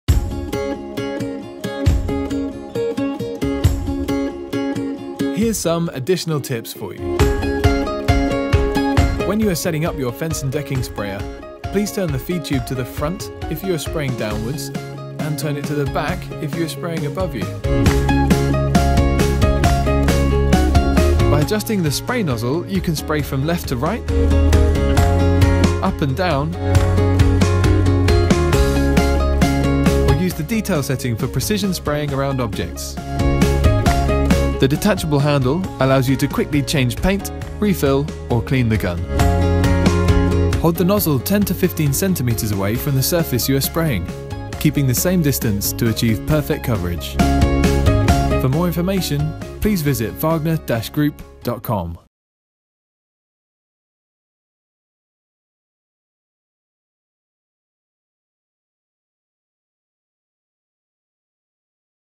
Wagner Fence & Decking Sprayer | Screwfix
With a fine spray system for quickly and easily applying solvent or water-based fence paint, oil and varnish, Wagner's Fence and Decking Sprayer is easily detachable when refilling and cleaning.